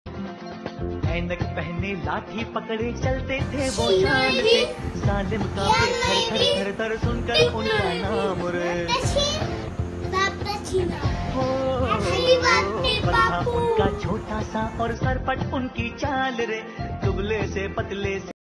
Gandhi Jayanti Celebration | See No Evil, Hear No Evil, Speak No Evil
They are too small to deliver a speech. It’s a very small tribute from them. Happy Gandhi Jayanti to you all.